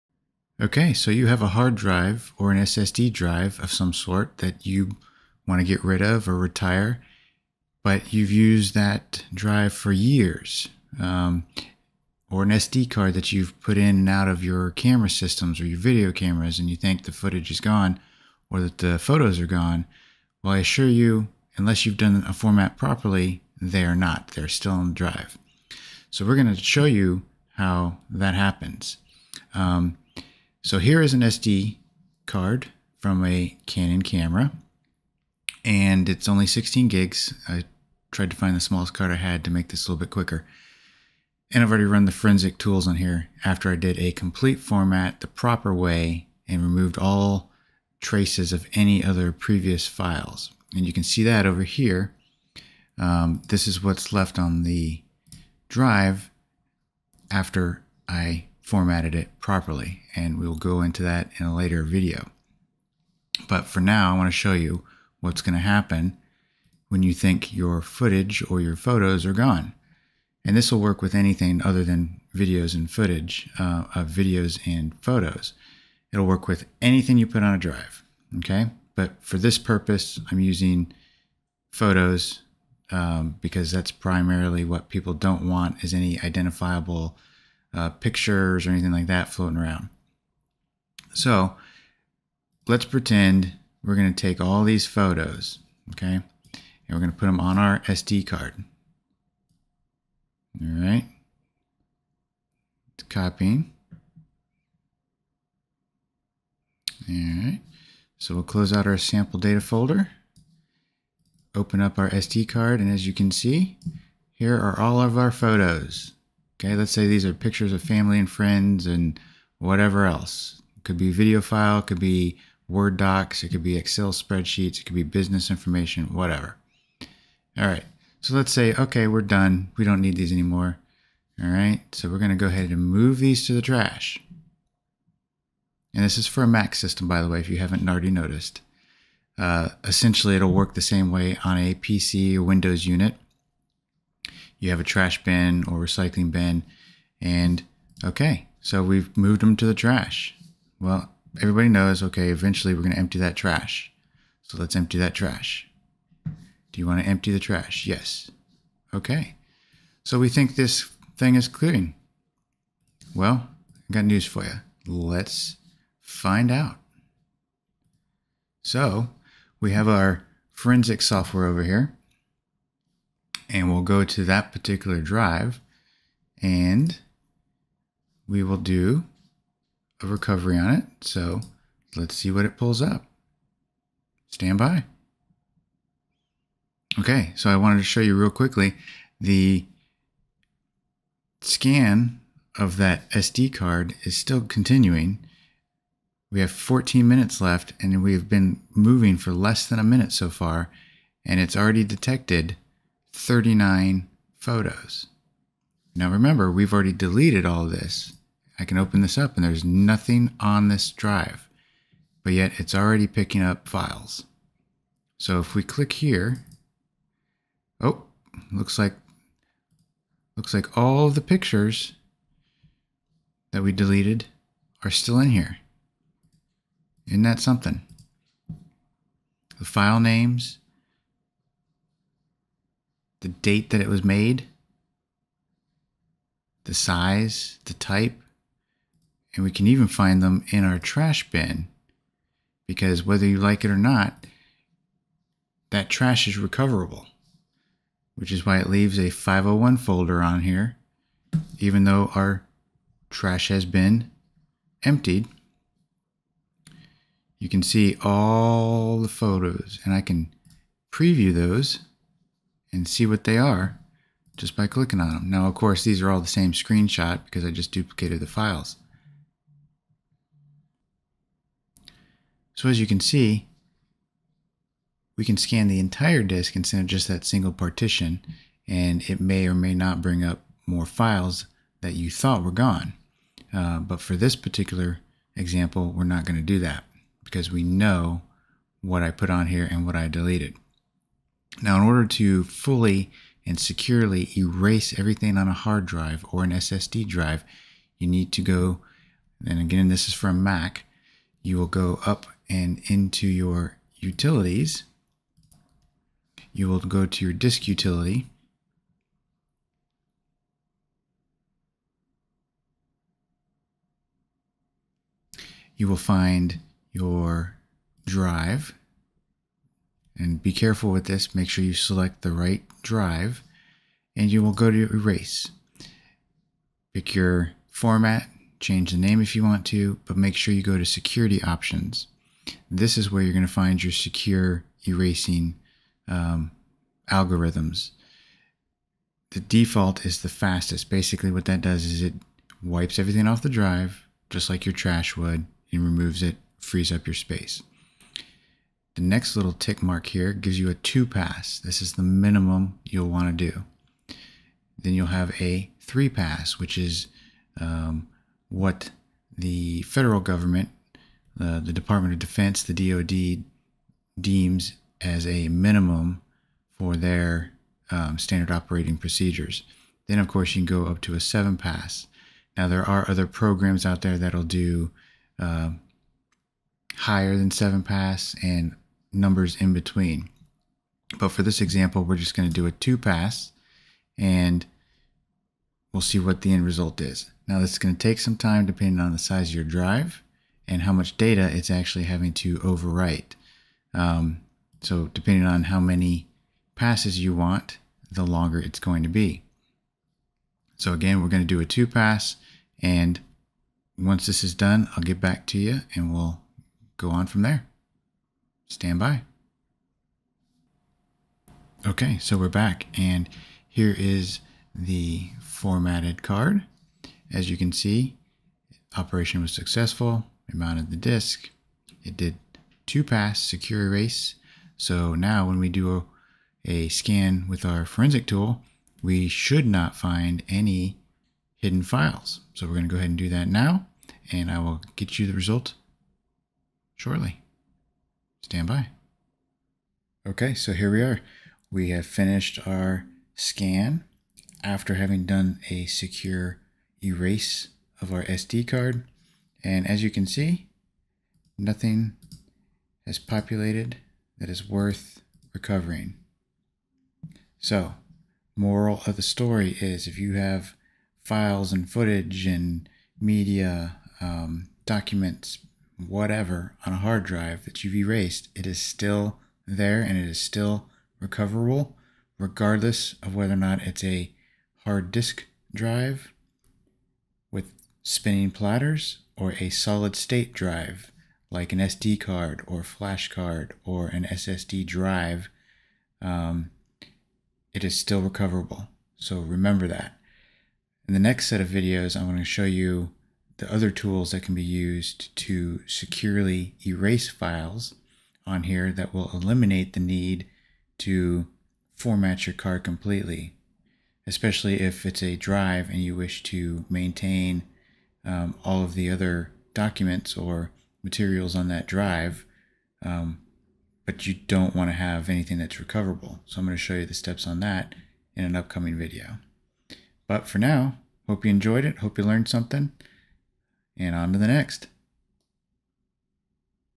Securely Erase a Drive (SSD/HDD) on a Mac - Remove Hidden Recoverable Files!
Think you've deleted the files because you've emptied the trash or FORMATTED that drive? Think again! If you don't physically destroy the drive or done a proper secure format on your drive, those files are recoverable. Even on those solid state drives, files CAN BE recovered!

Using a foresnic tool, I show you how easy it is to recover those files on an SD card (a form of SSD). Platter drives (HDD) are even easier to recover files from using lesser methods.

Don't sell your computers, hard drives, flash drives, etc. before doing a proper secure erase.